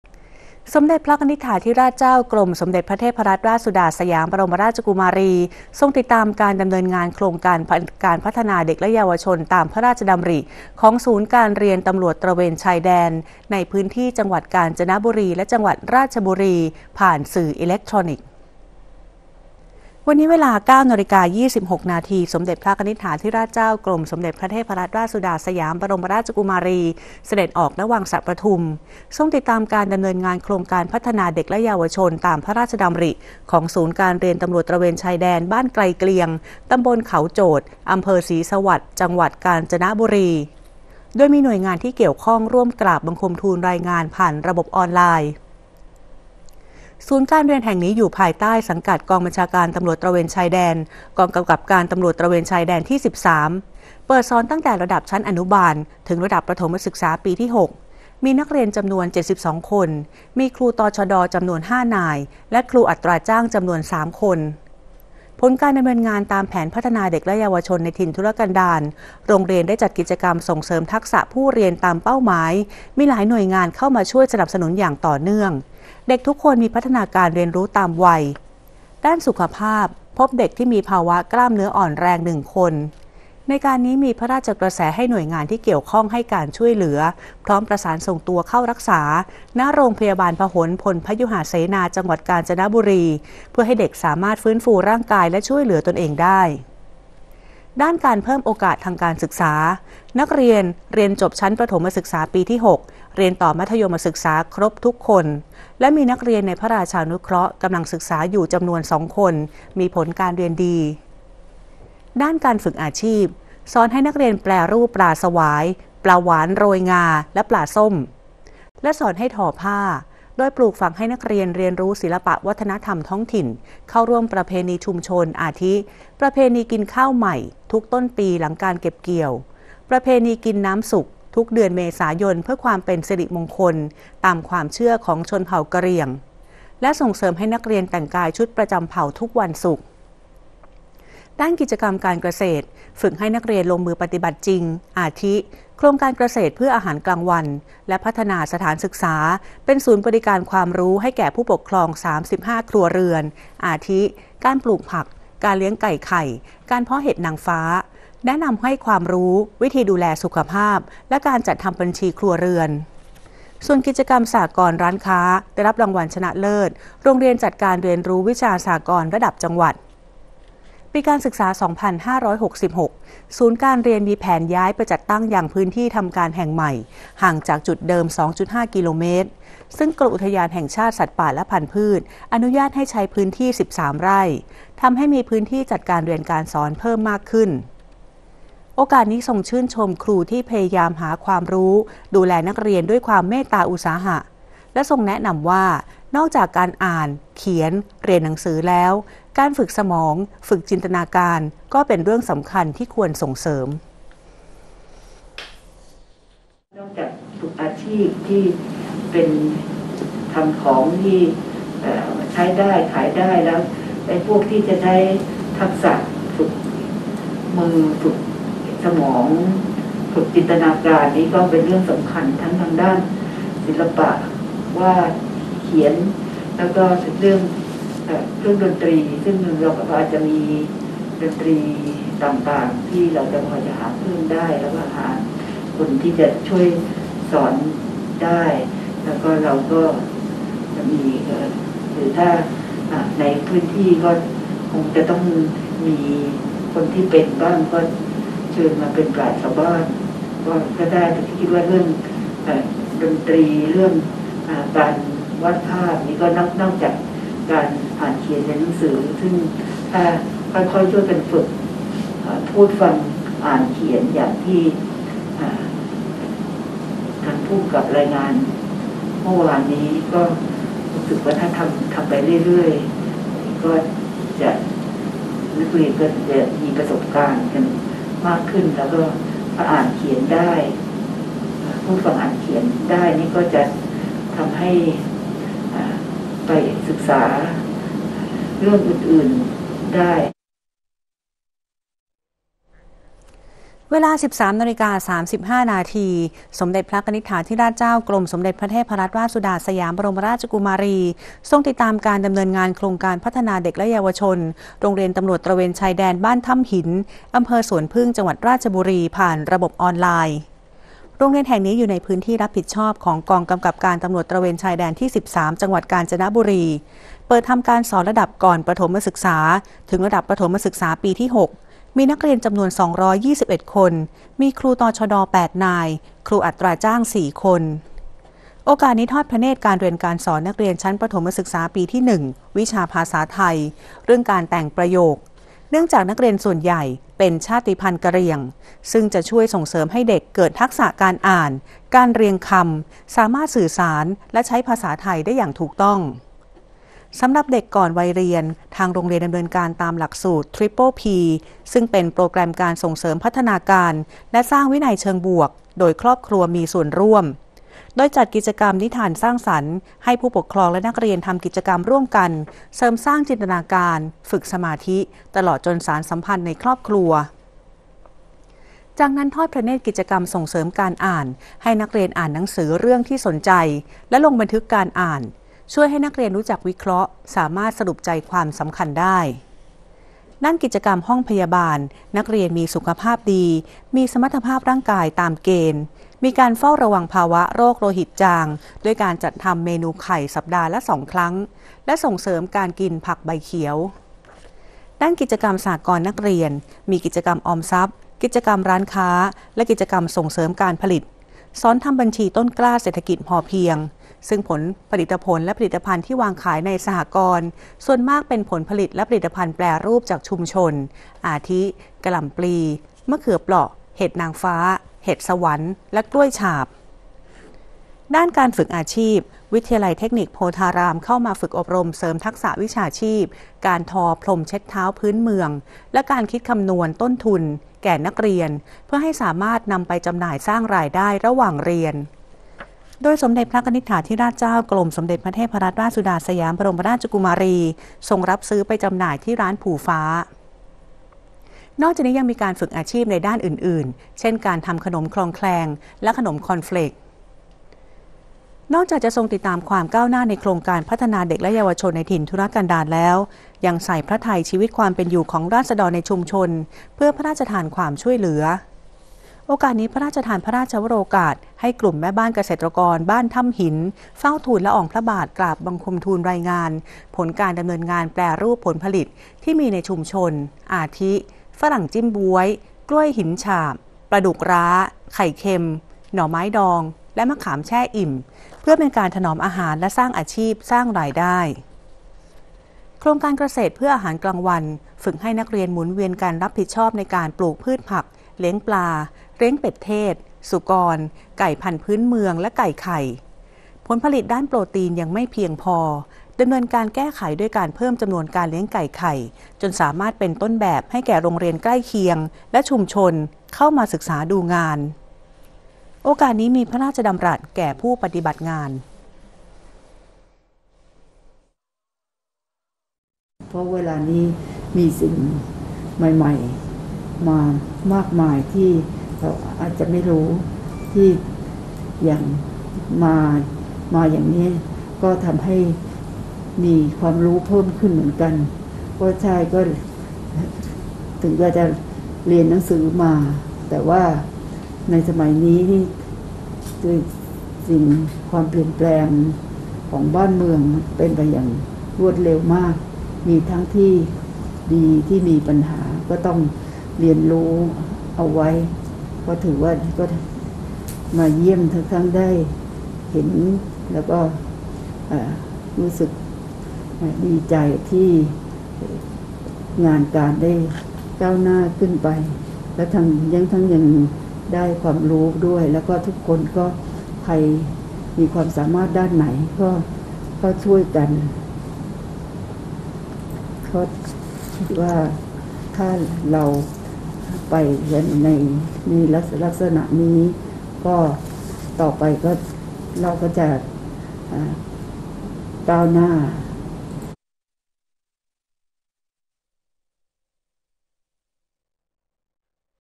กรมสมเด็จพระเทพฯ ติดตามโครงการพัฒนาเด็กและเยาวชนฯ ผ่านสื่ออิเล็กทรอนิกส์ วันที่ ๒๓ กุมภาพันธ์ ๒๕๖๖
สมเด็จพระกนิษฐาธิราชเจ้า กรมสมเด็จพระเทพรัตนราชสุดาฯ สยามบรมราชกุมารี ทรงติดตามการดำเนินงาน โครงการพัฒนาเด็กและเยาวชน ตามพระราชดำริ ของศูนย์การเรียนตำรวจตระเวนชายแดน ในพื้นที่จังหวัดกาญจนบุรี และ จังหวัดราชบุรี ผ่านสื่ออิเล็กทรอนิกส์